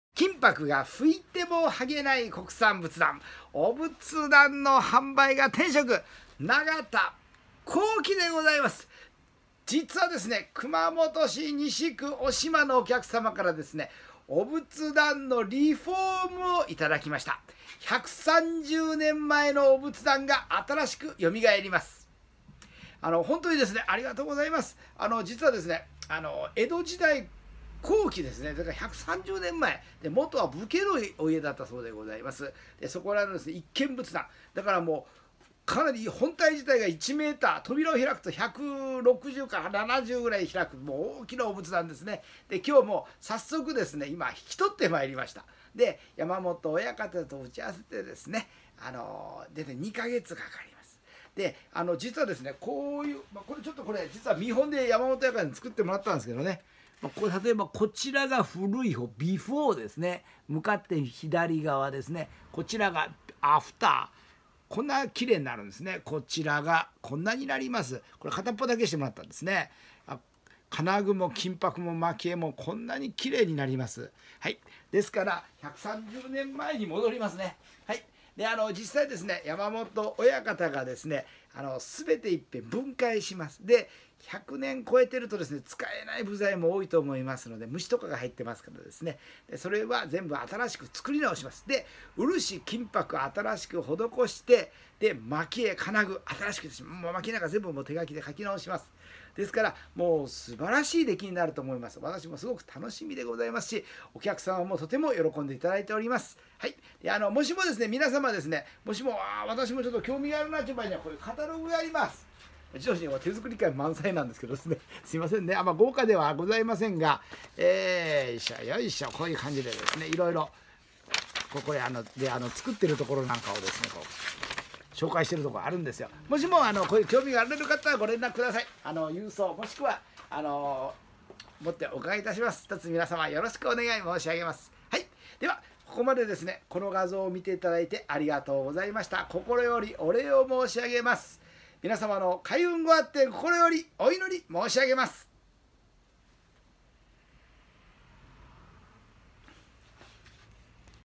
熊本 西区 小島 仏壇 リフォーム 再生 復元 おすす抜き 若返り 蘇り
熊本市西区小島のT様から、仏壇リフォームのご注文いただきました。
一度全て、山元親方が金箔・漆を剥いで分解いたします。使えない古い部材は、新しく作成いたします♬もう一度新しく漆を塗り、金箔を施します。金具・蒔絵も新しくします。
見事に、新品に蘇ります！
ご予算に応じて、安く対応いたします。
作業に2ヶ月お時間いただければ幸いです

朝５時〜夜7時一生懸命営業中♬
24時間受付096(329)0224生涯無休！